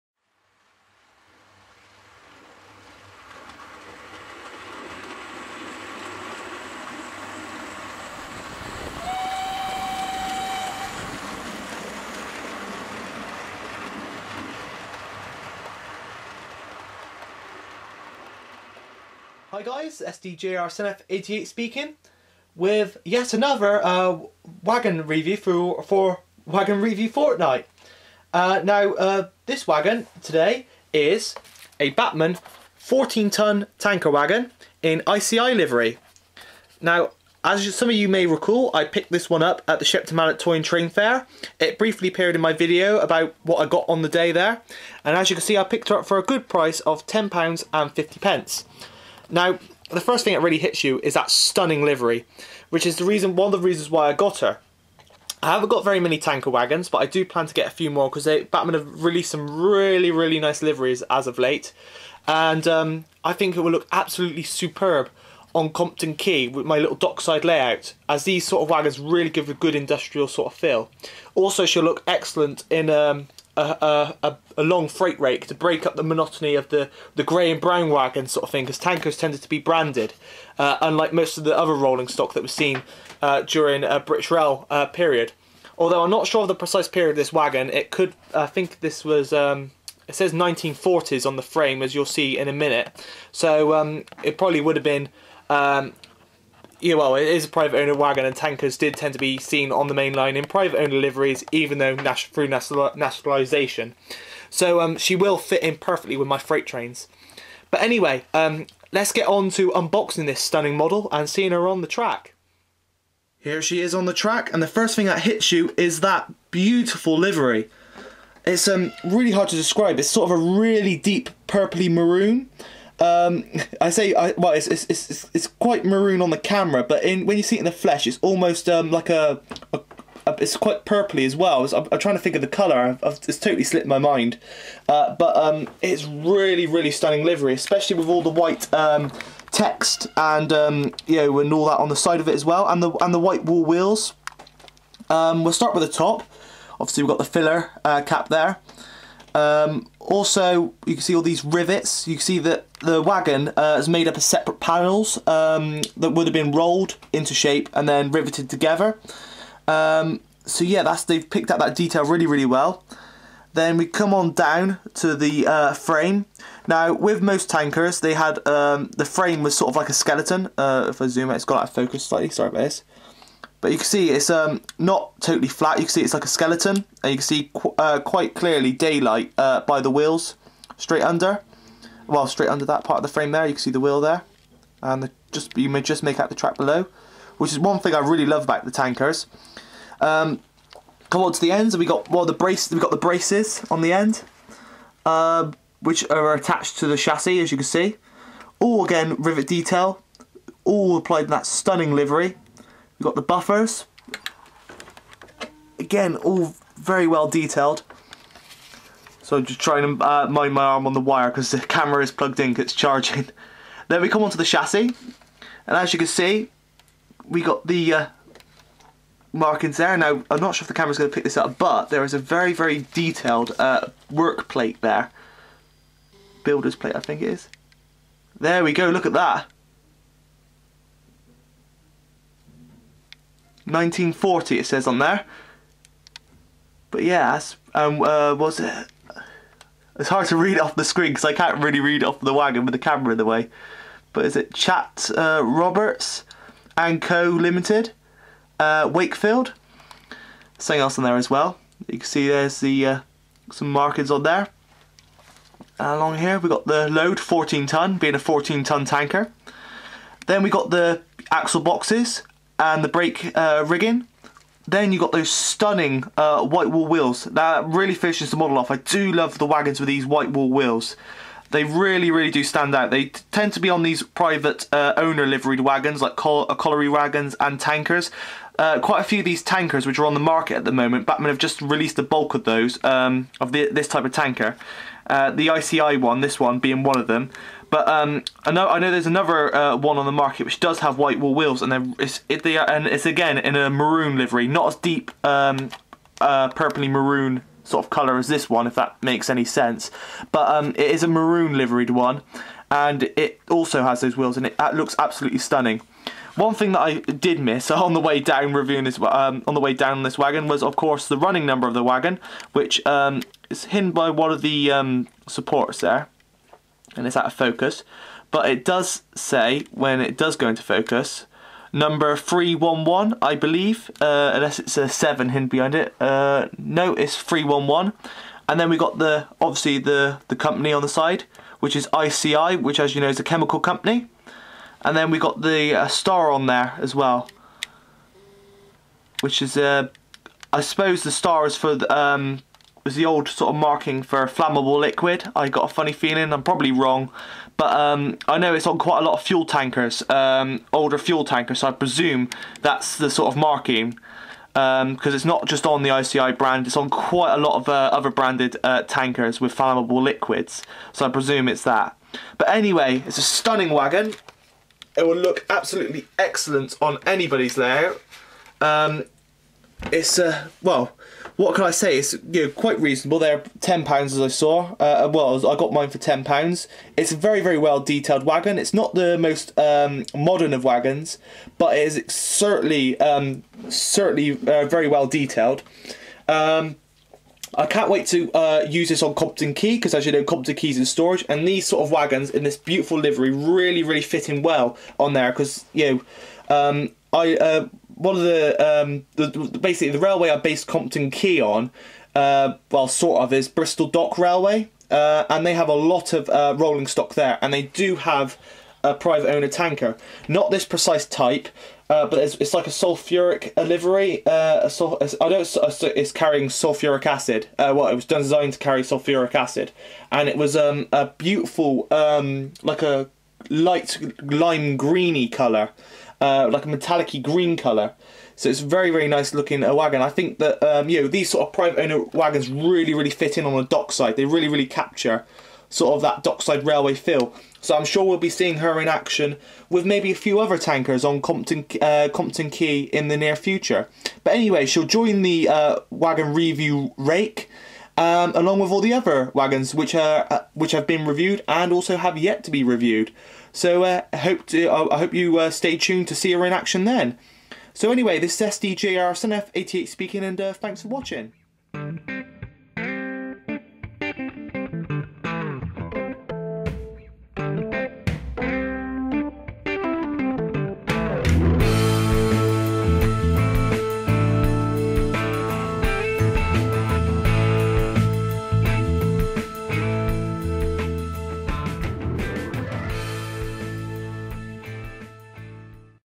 Bachmann 'ICI' 14 Ton Tanker Review, Ep 3
Another instalment of Wagon Review Fortnight! In this episode we look at the Bachmann 14 Ton Tanker in stunning 'ICI' livery. Avery lovely model indeed and will be sure to future on Compton Quay at some point too! Hope you enjoy!